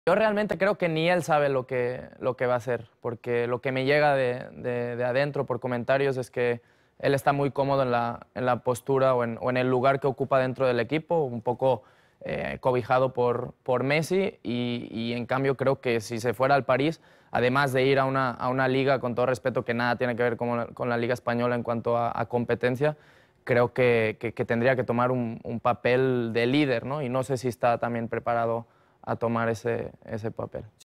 Opinión de experto sobre el traspaso de Neymar al PSG: “Ni él tiene claro lo que quiere hacer”
Nuestro analista Marc Crosas, muy cercano al equipo catalán, aseguró que la situación que vive el atacante brasilero del FCB es complicada, al no haber tomado ninguna decisión sobre la oferta del PSG.